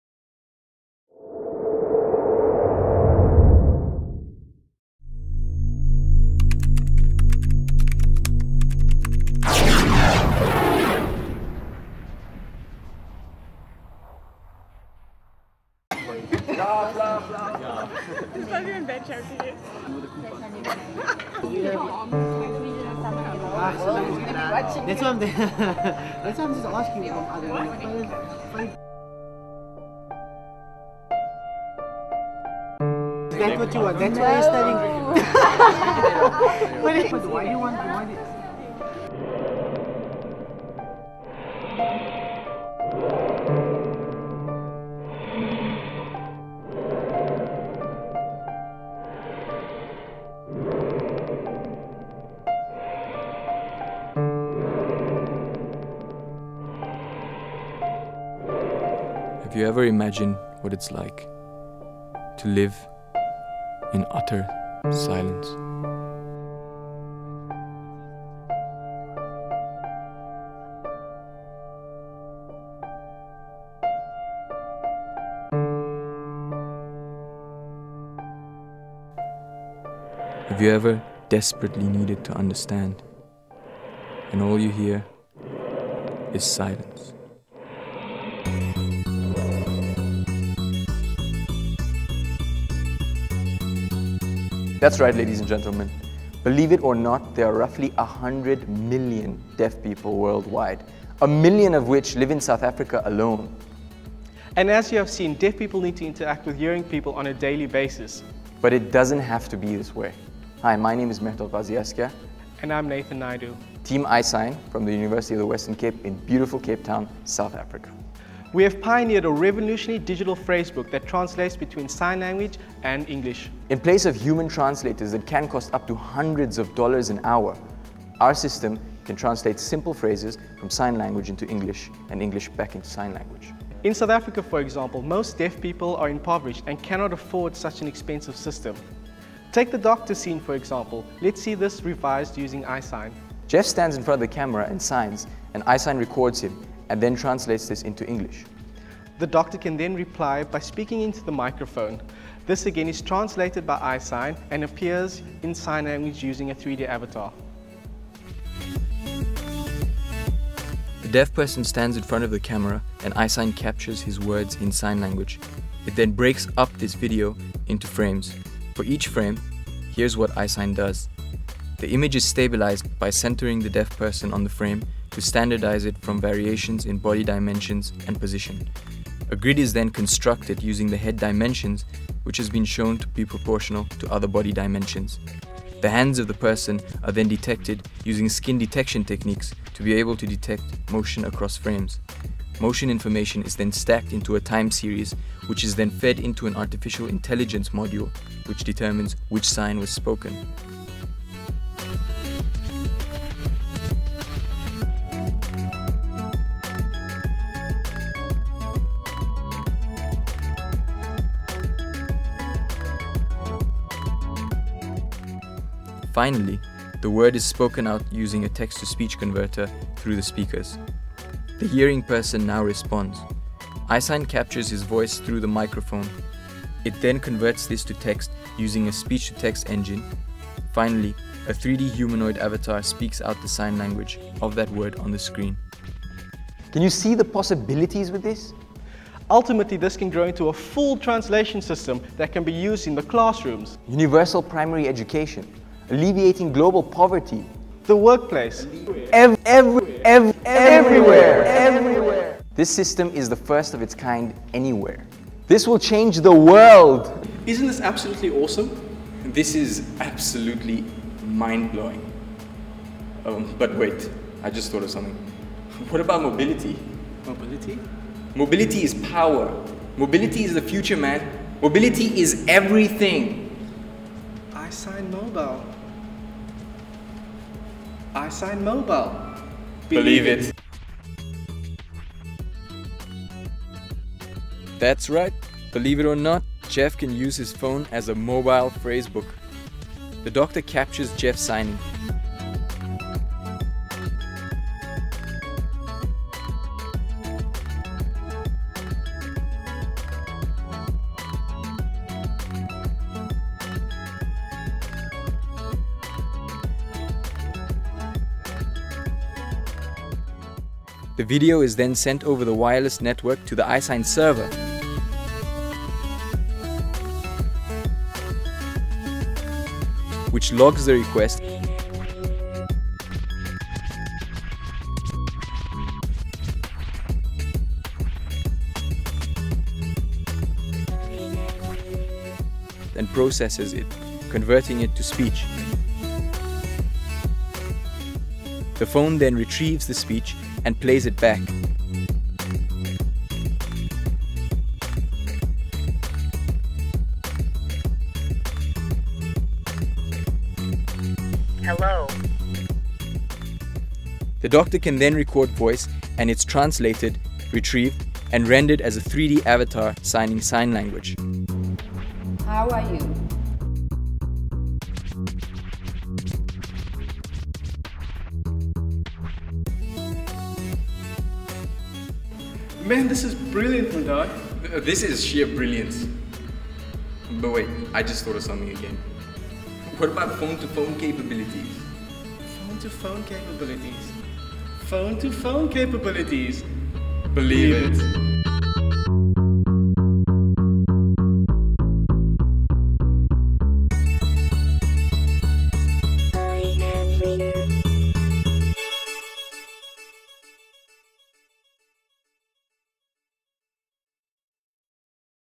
iSign.mov
2009 Project with two Masters computer Science students at UWC which was submitted for a competition on innovative ideas. Not sure what has happened to this invention.